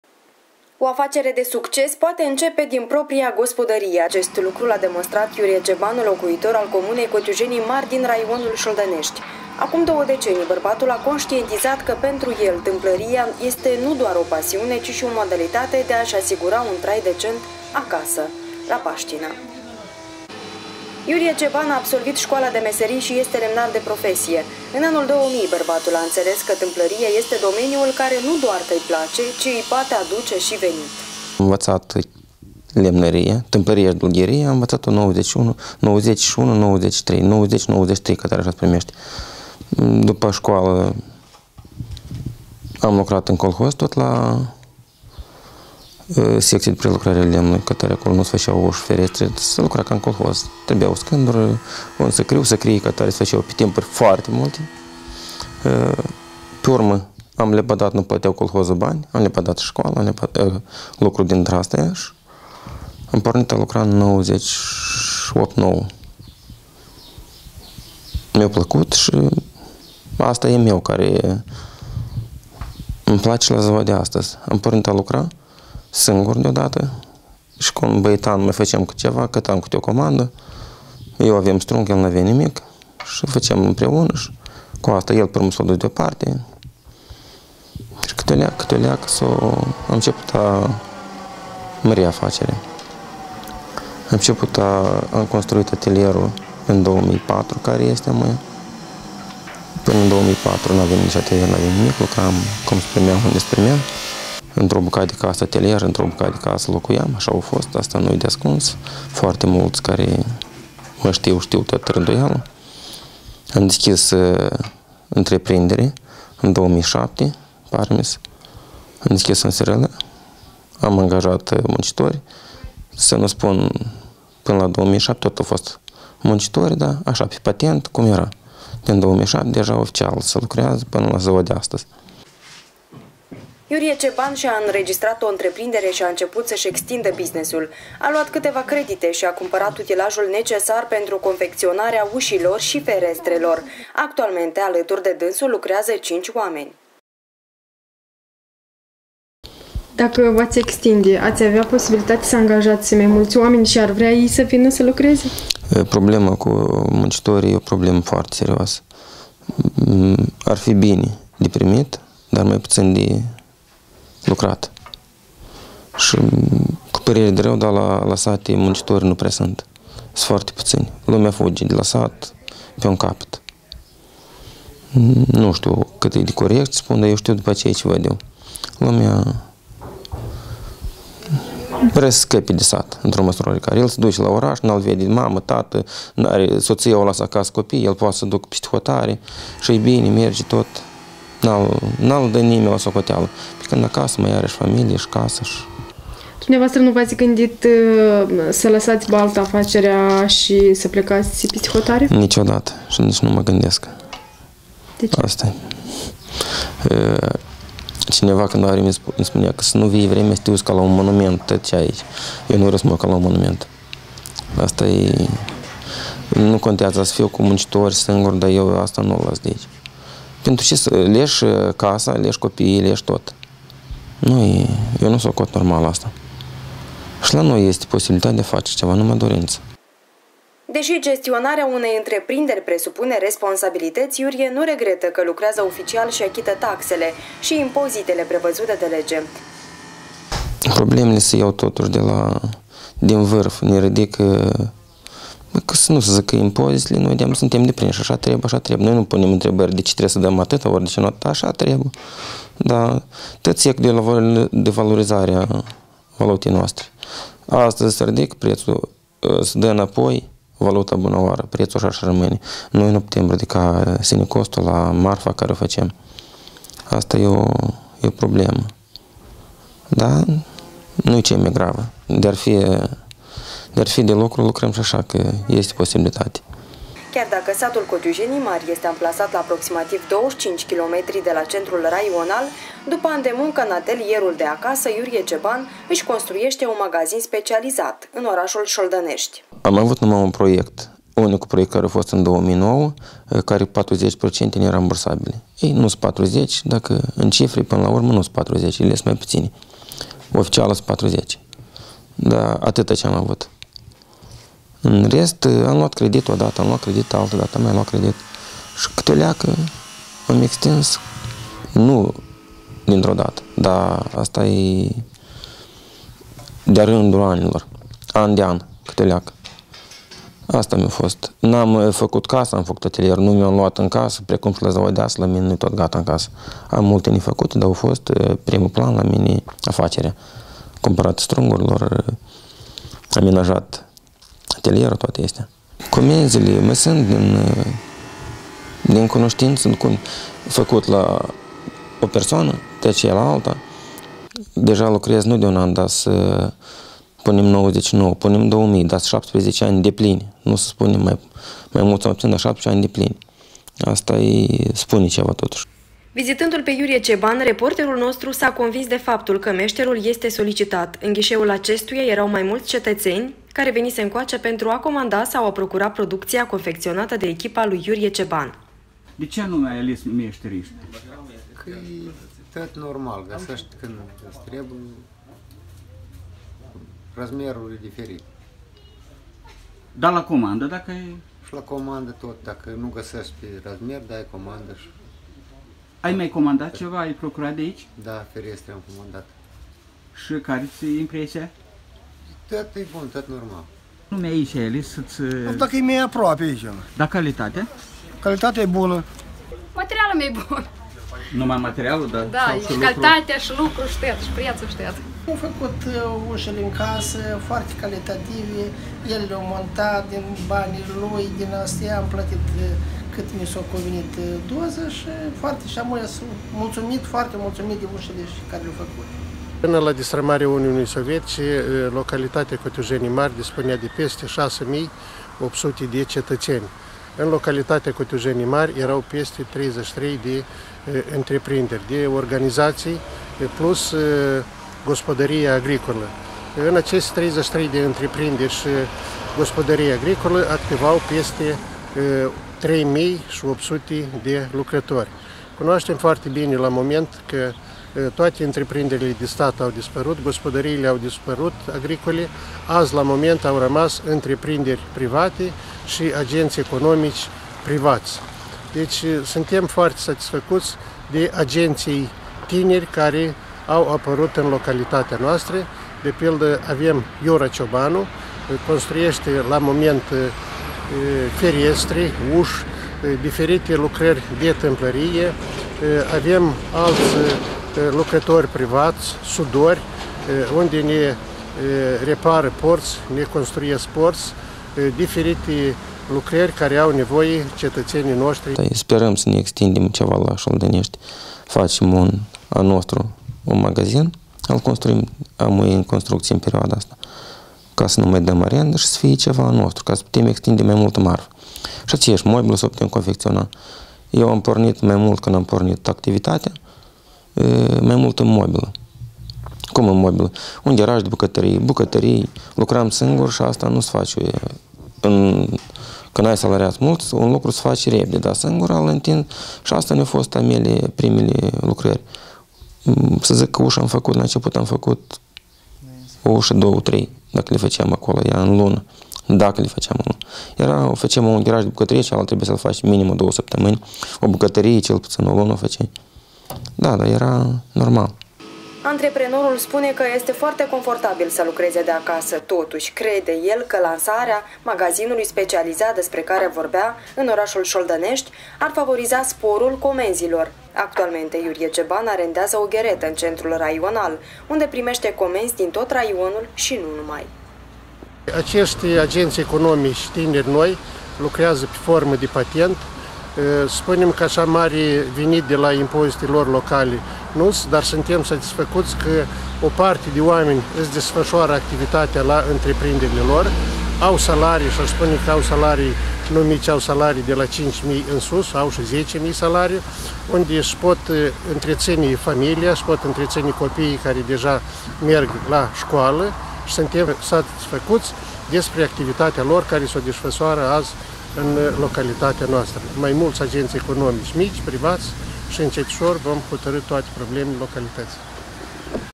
МАСТЕРСКАЯ В СОБСТВЕННОМ ДОМЕ
Житель Шолданешт оборудовал с своём доме столярную мастерскую и занялся бизнесом. История успеха от коллег из студии Impuls tv.